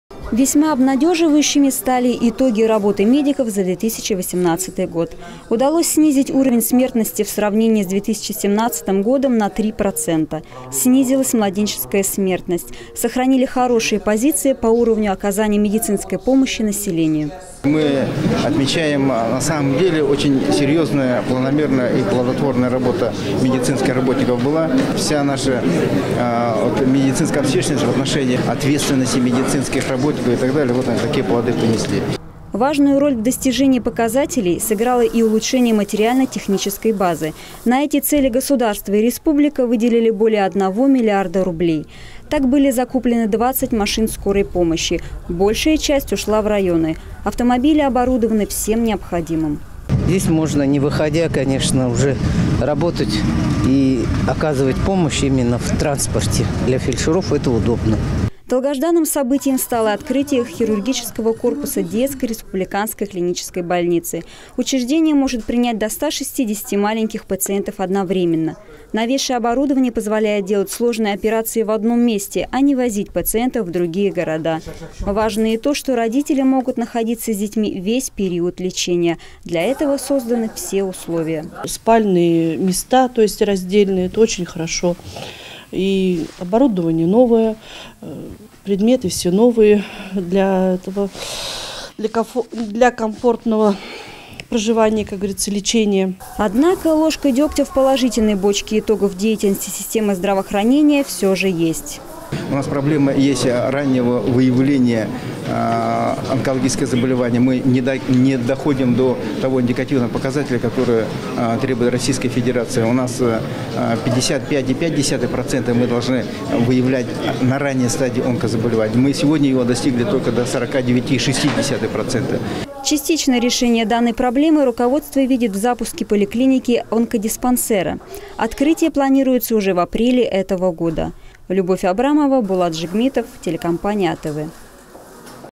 Минздрав Бурятии подвёл итоги 2018 года
В системе здравоохранения республики накануне подвели итоги деятельности за 2018 год.Руководители отчитались о проделанной работе. Обсудили проблемы. Наметили планы на будущее.  Подробнее, смотрите  в сюжете.